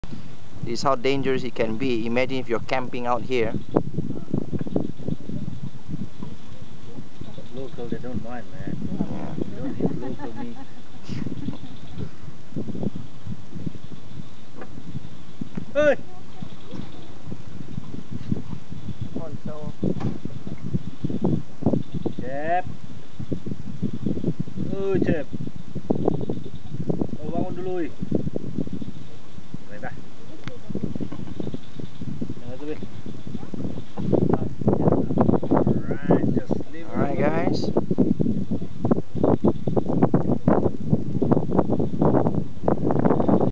Kinabatangan River: Saltwater Crocodile (Crocodylus porosus) basking on the bank.
Found this guy basking in the afternoon sun as we were heading downriver. Crocs, like other reptiles depend on the heat from the sun to raise their core temperature so they can go about their business. And also for things like digesting the tourist they had last night :-D